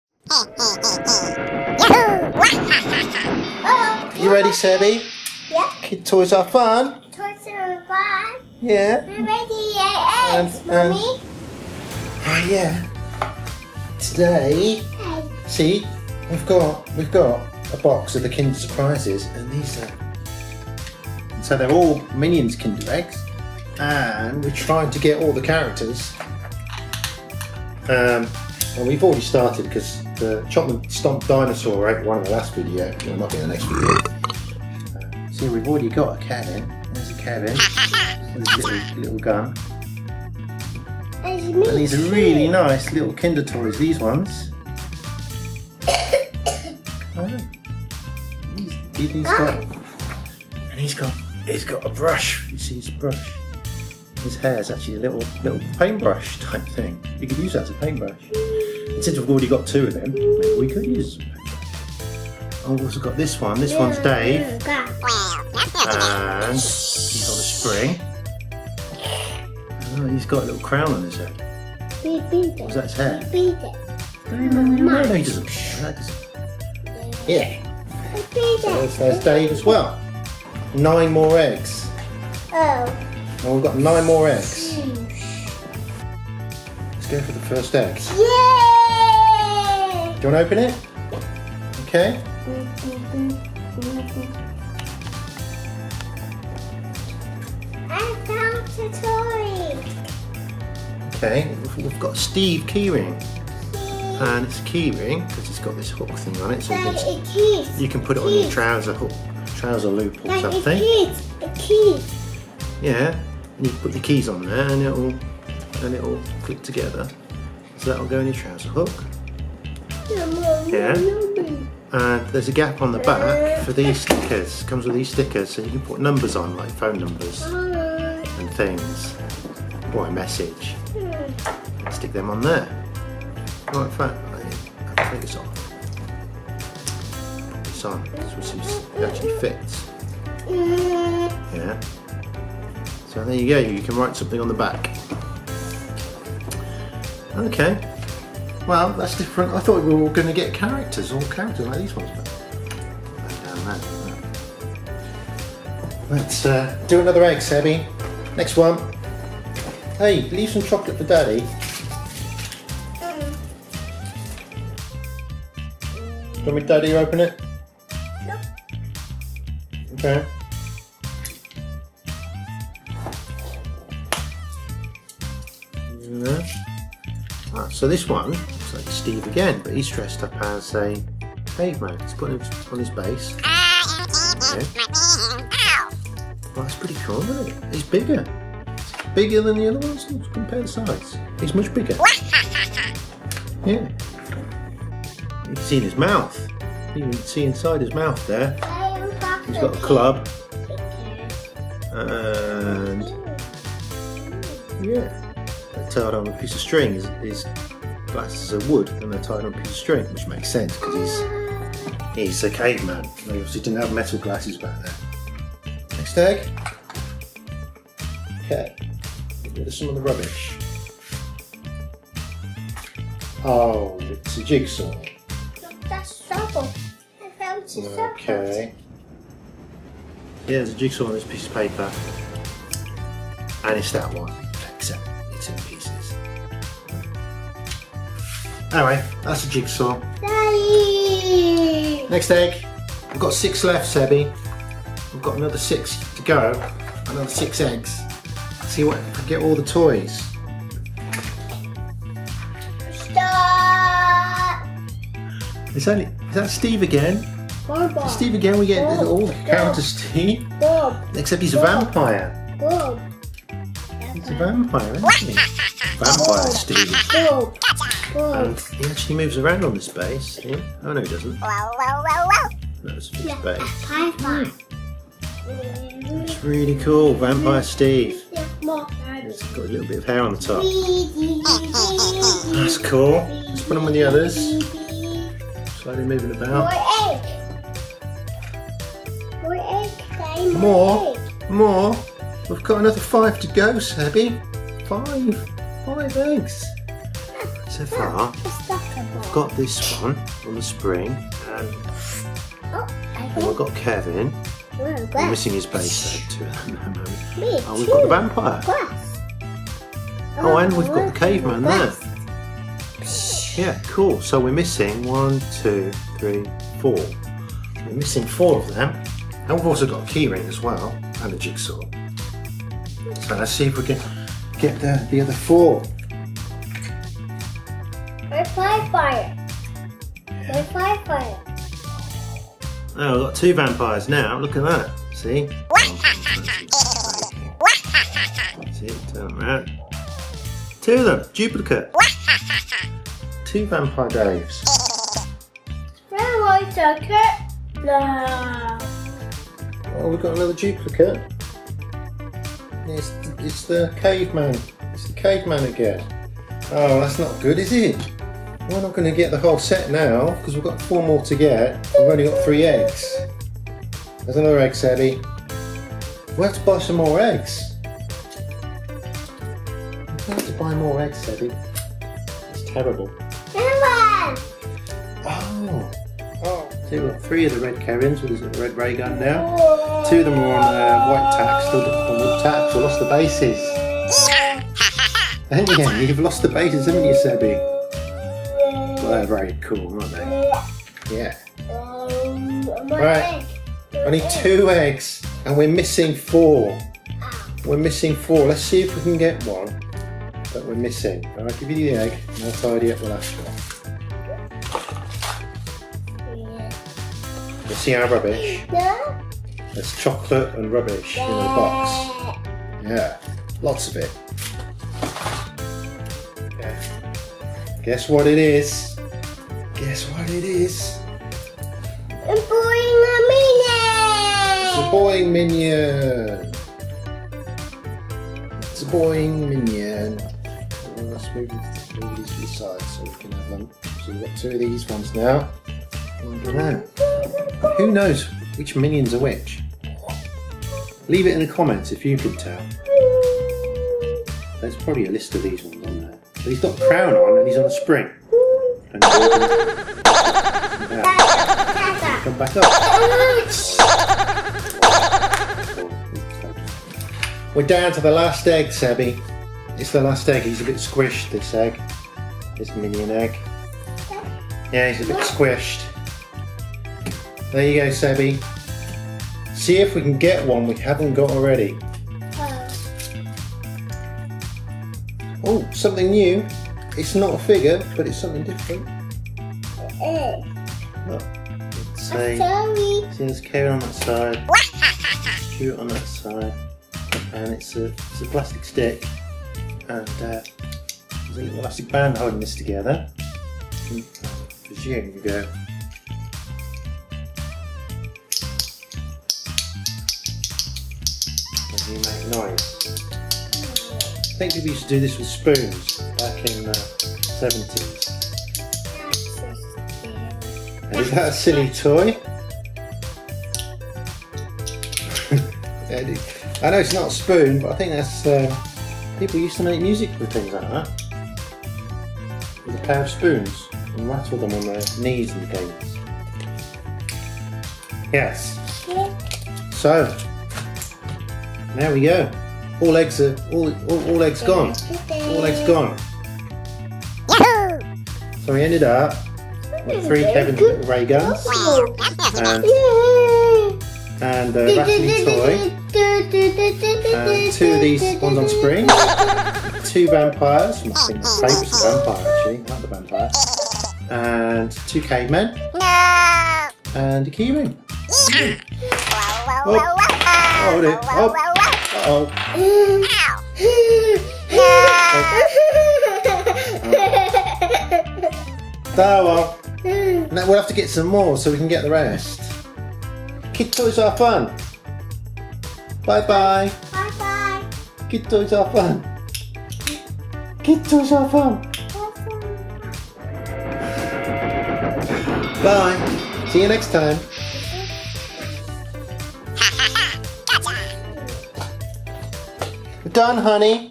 Minions Kinder Surprise Eggs - Can we get the complete Minion toy set?
Nine new Kinder Surprise eggs and three opened eggs, we are trying to see if we can get all the Minion toys, will it work? How many duplicates will there be? Who knows!!! Only one way to find out, it's hard work but someone has to open all those eggs and somehow eat all that chocolate.

Gav.
KTAF - Kid Toys Are Fun

5 DUPLO Deluxe Train Sets - Kid Toys Are Fun "